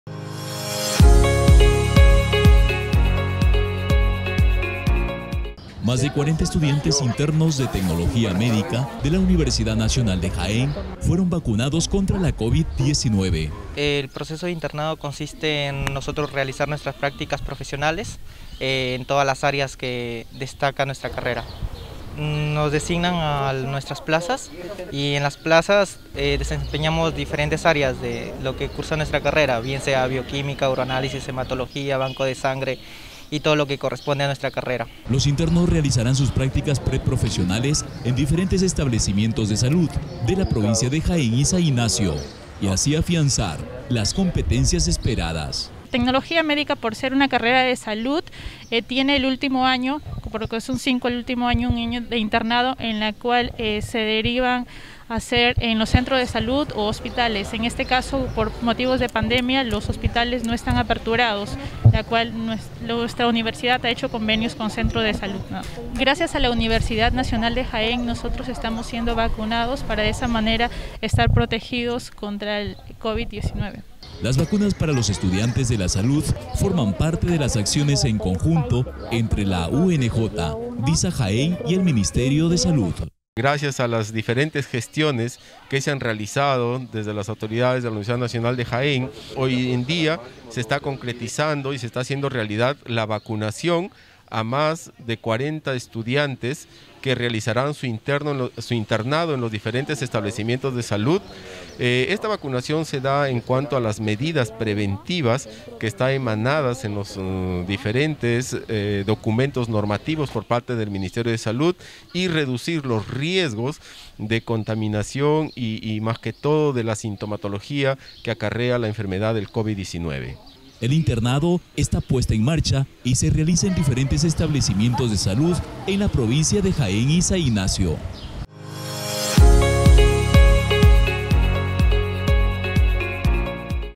INTERNOS DE TECNOLOGÍA MÉDICA RECIBIERON LA PRIMERA DOSIS DE VACUNA CONTRA LA COVID-19.
Más de 40 estudiantes de Tecnología Médica de la Universidad Nacional de Jaén (UNJ) recibieron la vacuna contra la COVID-19 y así desarrollar sus actividades académicas en los distintos establecimientos de salud de Jaén y San Ignacio.

Según informó el coordinador de Tecnología Médica, mag. Enrrique Arellano Ubillus, este es el resultado de las gestiones en conjunto entre la UNJ la Dirección de Salud (DISA) de Jaén y el Ministerio de Salud (MINSA), y, para el próximo 31 de mayo se realizará la inmunización a los estudiantes que realizarán su internado en la provincia de San Ingnacio.

NOTA:
Según información del MINSA, hasta el momento, Perú ha recibido un millón de vacunas de Sinopharm, tres millones 195 mil dosis dosis de Pfizer y cerca de 400 mil del mecanismo Covax Facility (de AstraZeneca y Pfizer). Todas estas se aplican a personal de salud, adultos mayores, miembros de la Policía Nacional, Fuerzas Armadas y Bomberos, comprendidos en la primera fase del Plan Nacional de Vacunación.